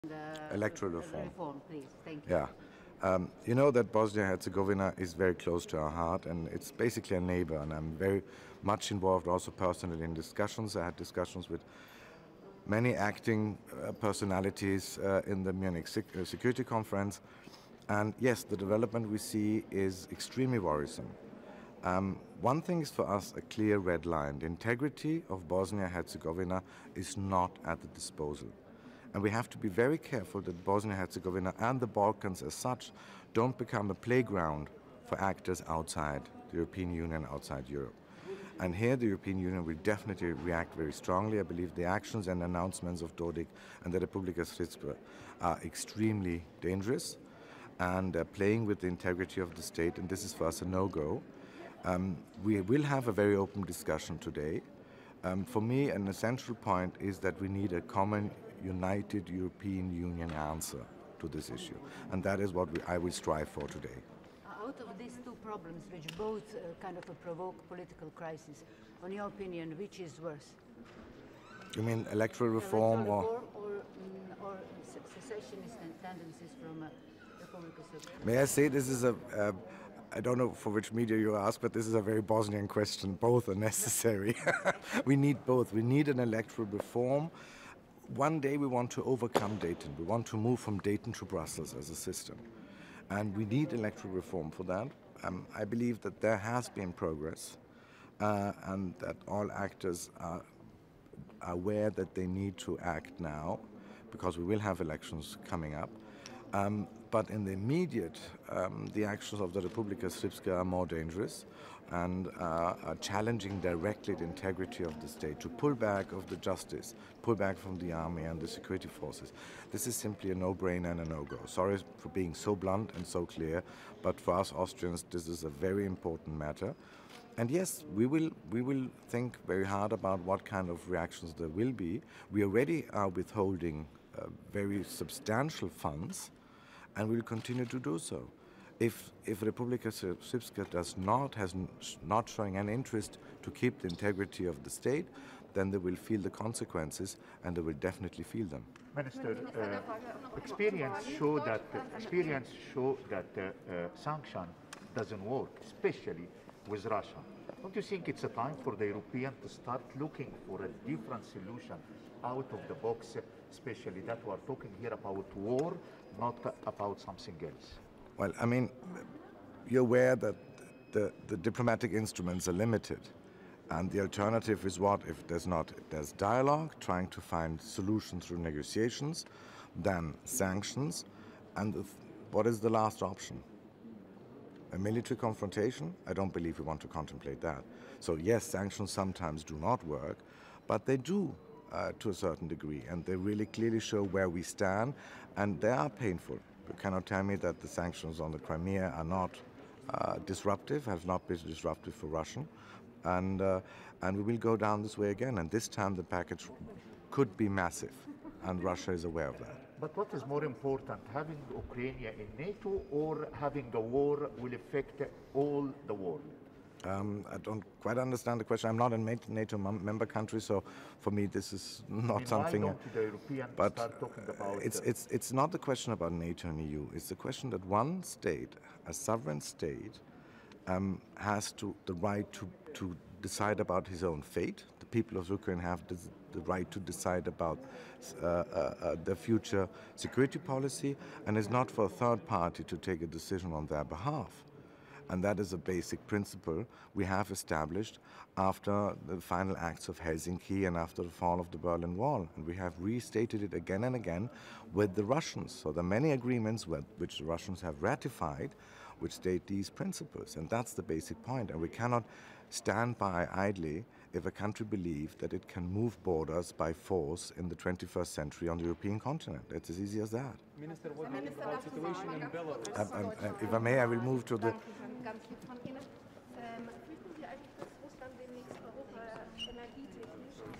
EU mulls ways to stop the possible breakup of Bosnia! Alexander Schallenberg warns!
Austrian Foreign Minister Alexander Schallenberg said that “the announcements of Dodik and Republika Srpska are extremely dangerous and playing with the integrity of the state, and this is for us a no-go.”

He warned that Bosnia and the Balkans must not become “a playground for actors outside Europe.” Russia has notably pledged support for Dodik and his associates.

The EU is also demanding that electoral reform be passed ahead of polls in Bosnia later this year.

What the EU wants to see, Irish Foreign Minister Simon Coveney said, is “a full reform package agreed and implemented in advance of the elections so that we can have elections that then successfully result in the formation of a government that can function.”

"We want to see peace and stability in Bosnia and Herzegovina, and I think that reform package would certainly help that to take place,” Coveney told reporters.

European Union foreign ministers on Monday discussed ways to ease tensions in Bosnia and prevent the possible breakup up of the ethnically divided Balkan country as the peace agreement brokered over 25 years ago continues to unravel.

“The nationalist and separatist rhetoric is increasing in Bosnia and Herzegovina and jeopardizing the stability and even the integrity of the country,” EU foreign policy chief Josep Borrell said in Brussels, where he chaired a meeting of the 27-country bloc’s foreign ministers.

He said the “ministers will have to take a decision on how to stop these dynamics in Bosnia and Herzegovina and to avoid that the country can fall apart in pieces. This is a critical situation.”

The United States last month announced new sanctions against Bosnian Serb leader Milorad Dodik, who has for years been advocating that the Serb-run part of Bosnia should leave the rest of the country and unite with neighboring Serbia.

The U.S. accused him of “corrupt activities” that threaten to destabilize the region and undermine the U.S.-brokered Dayton Peace Accord. Dodik says he and Bosnian Serbs are being unfairly targeted and wrongly accused of corruption.

The agreement in 1995 ended the war in Bosnia, which killed more than 100,000 people and left millions homeless. The accord established two separate governing entities in Bosnia — Republika Srpska run by Bosnia’s Serbs, and another other dominated by Bosniaks, an ethnic group that is primarily Muslim, and Croats. The two are linked by shared, state-wide institutions, and all actions at a national level require consensus from all three ethnic groups.

Most EU countries also want to hit Dodik with sanctions, but Hungary, Croatia and Slovenia are notably opposed and likely to scupper any attempt to impose restrictive measures on him.